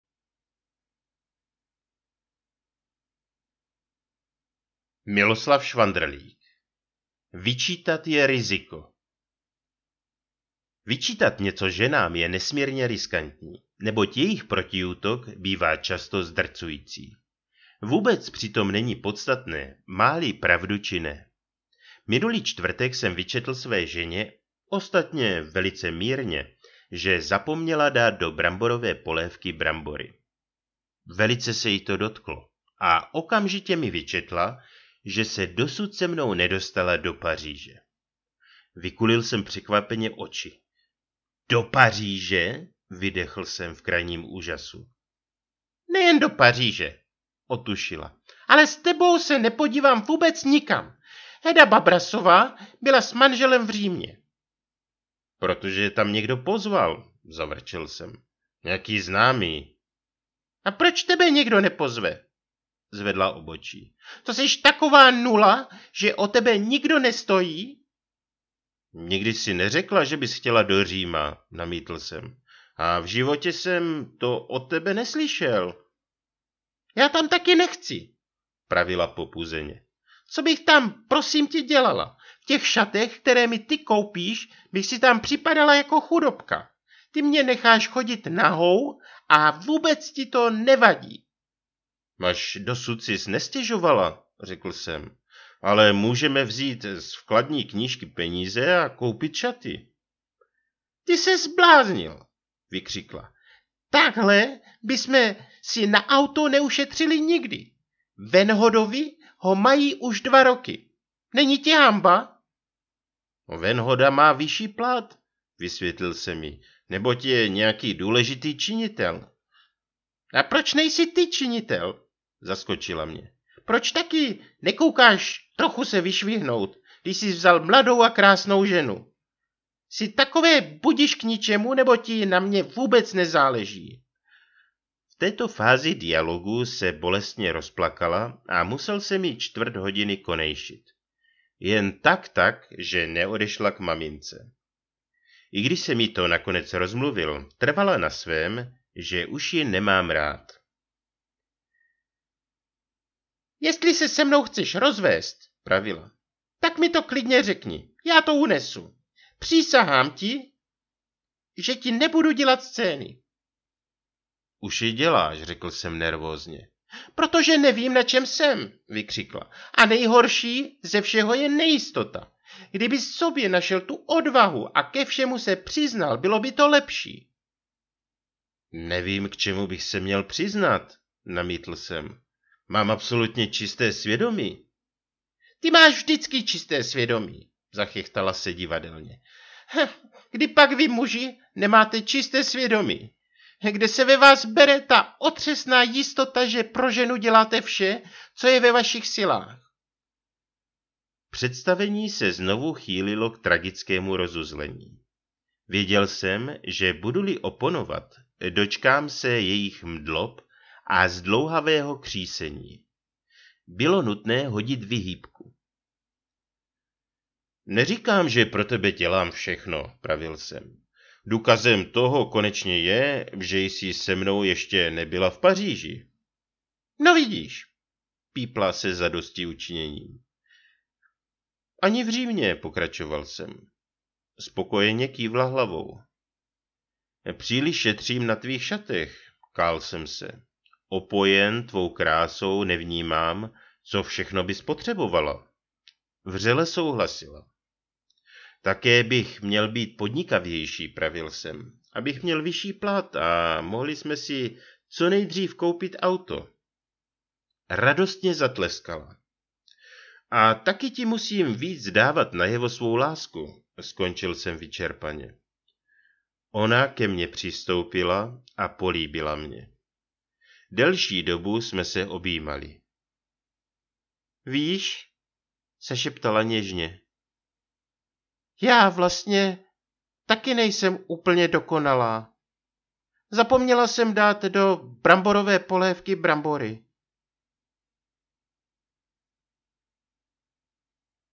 Švandrlík Miloslav - VYČÍTAT JE RIZIKO!
VYČÍTAT JE RIZIKO! - povídka MILOSLAVA ŠVANDRLÍKA
z knihy SEXBOMBA NA DOPLŇKOVOU PŮJČKU, vydala Česká expedice a Nakladatelská společnost RICHTER jako svou první publikaci edice DUHA v roce 1991.
Ilustrace: Jiří Winter - Neprakta
čte: T. Texas Taylor